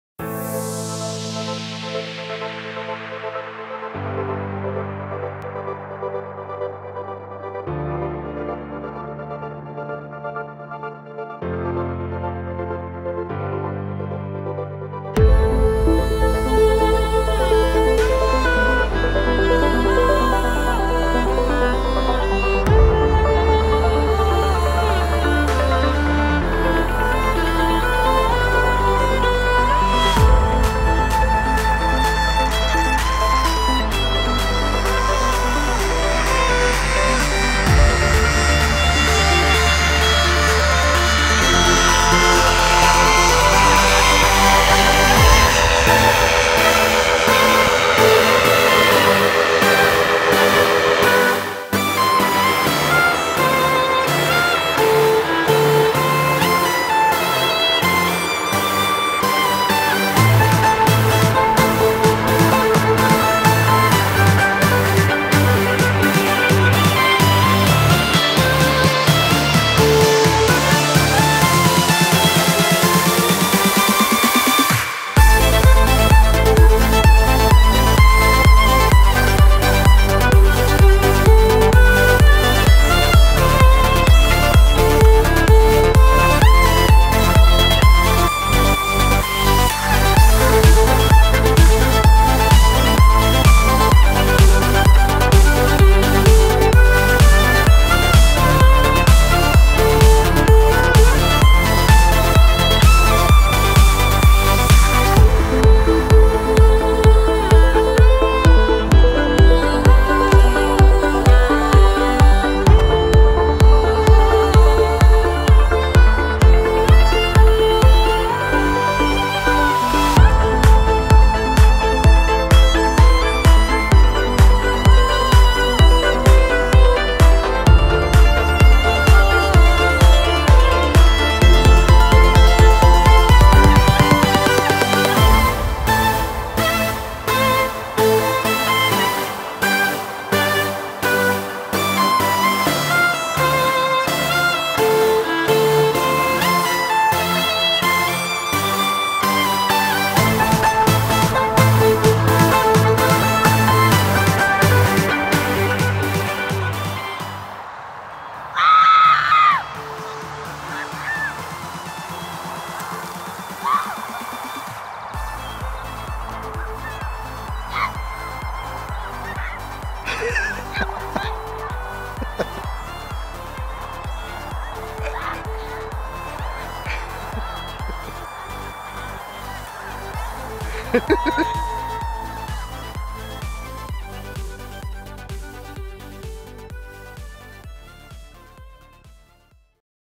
Yannie's first solo flight!!!
Yannie's first solo paramotor flight on June 24th 2016! Congratulations!!... Yeehaa!!...